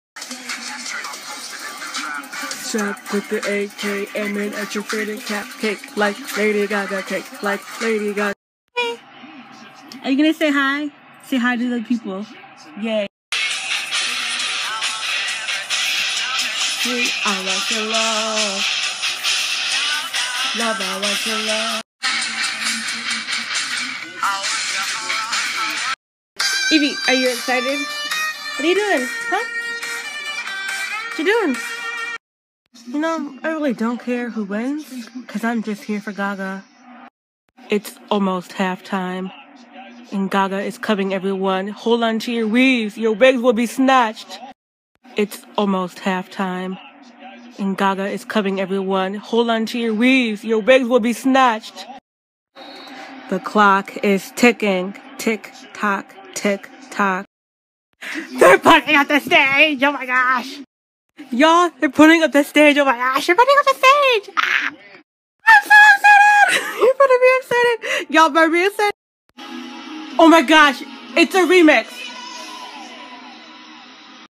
Gaga Pre Halftime Freakout
This is prior to Gaga's performance of my freakout until she jumps off the roof the next 2 videos will be my reaction 😂